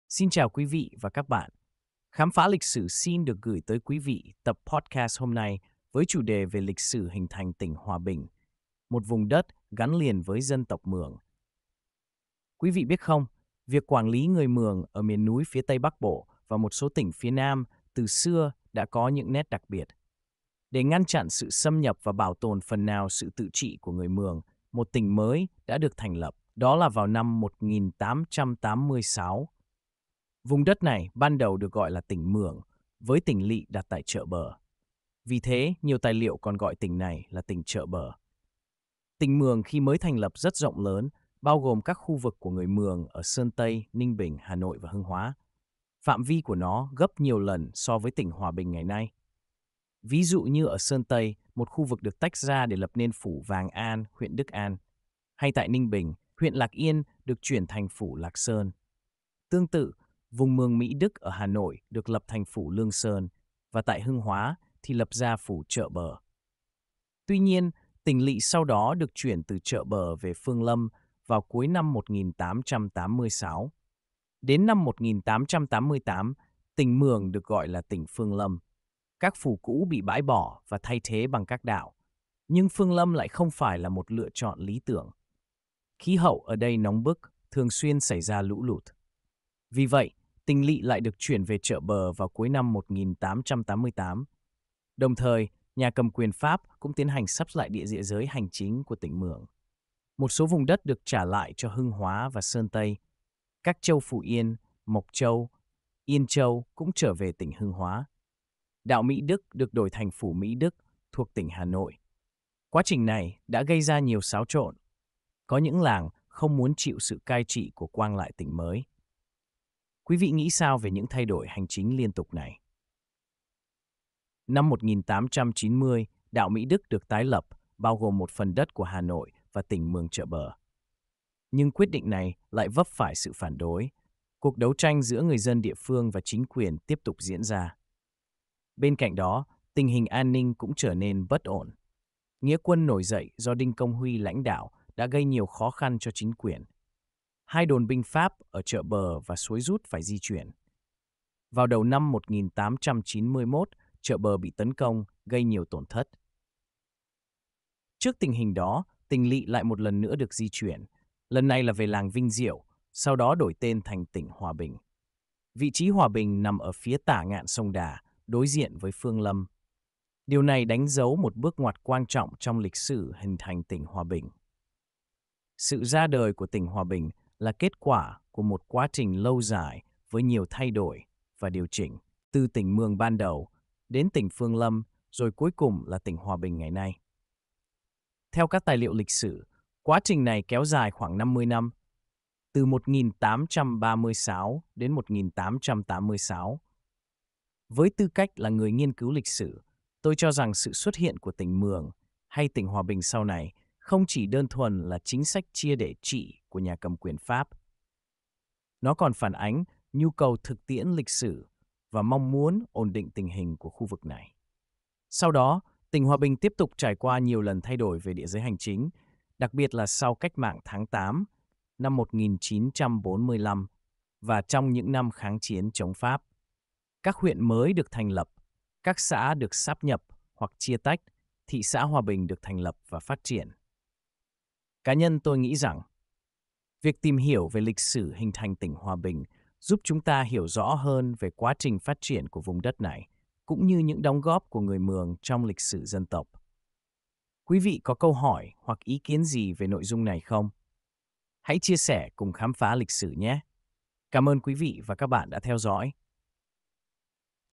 Hành Trình Hình Thành Tỉnh Hòa Bình: Từ Mường Đến Núi
id171983 - Hành Trình Hình Thành Tỉnh Hòa Bình: Từ Mường Đến Núi

Vùng đất phía Tây Bắc Bộ, nơi sinh sống của đồng bào Mường từ xa xưa, đã trải qua một hành trình dài và đầy biến động để trở thành tỉnh Hòa Bình như ngày nay. Bài viết này sẽ lần giở lại những dấu mốc quan trọng trong quá trình hình thành và phát triển của đơn vị hành chính đặc biệt này, từ thời kỳ phong kiến đến hiện đại.
Bản đồ Bắc Kỳ cuối thế kỷ 19
Kể từ thời phong kiến, cộng đồng người Mường đã được đặt dưới một chế độ hành chính riêng biệt. Nhận thấy tầm quan trọng chiến lược của vùng đất này, cũng như mong muốn duy trì sự ổn định và bảo tồn nền tự trị của các xứ Mường, Kinh lược sứ Bắc Kỳ Nguyễn Trọng Hợp đã đề xuất thành lập tỉnh Mường.  Ngày 22 tháng 6 năm 1886, Nghị định về việc thành lập tỉnh Mường chính thức được ký, đánh dấu một bước ngoặt lịch sử. Tỉnh lỵ ban đầu được đặt tại Chợ Bờ, thuộc châu Đà Bắc, tổng Hiền Lương.
Sự Ra Đời Của Tỉnh Mường và Những Thay Đổi Ban Đầu
Tỉnh Mường lúc mới thành lập có quy mô rộng lớn, bao gồm các vùng đất người Mường sinh sống tại Sơn Tây, Ninh Bình, Hà Nội và Hưng Hóa. Cụ thể, tỉnh được chia thành 4 phủ: Vàng An (Sơn Tây), Lạc Sơn (Ninh Bình), Lương Sơn (Hà Nội) và Chợ Bờ (Hưng Hóa). Quy mô này lớn hơn nhiều so với tỉnh Hòa Bình sau này. Tuy nhiên, chỉ vài tháng sau, ngày 29 tháng 11 năm 1886, tỉnh lỵ đã được di chuyển từ Chợ Bờ về Phương Lâm, tổng Hoằng Nhuệ, phủ Vàng An.
Sự thay đổi này không phải là cuối cùng. Tháng 4 năm 1888, tỉnh Mường được đổi tên thành tỉnh Phương Lâm, cấp phủ bị bãi bỏ và thay thế bằng các đạo. Tuy nhiên, do điều kiện tự nhiên không thuận lợi ở Phương Lâm, tỉnh lỵ lại được chuyển về Chợ Bờ vào cuối năm 1888.  Sự dịch chuyển liên tục của tỉnh lỵ cho thấy những khó khăn trong việc tìm kiếm một vị trí phù hợp cho trung tâm hành chính của tỉnh mới.
mai-chc3a2u-te1bb89nh-hc3b2a-bc3acnh.jpgĐèo Thung Khe, thị trấn Mai Châu, tỉnh Hòa Bình
Việc tái lập đạo Mỹ Đức và chia nhỏ tỉnh Mường Chợ Bờ năm 1890 đã gây ra sự phản đối mạnh mẽ từ phía người dân địa phương và các quan lại. Các quan lang người Mường khẳng định vùng đất của họ phải là một tỉnh độc lập.  Những mâu thuẫn này cho thấy sự phức tạp trong việc quản lý hành chính và sự khác biệt văn hóa giữa người Mường và các dân tộc khác.
Từ Phương Lâm Đến Hòa Bình: Định Hình Mộ...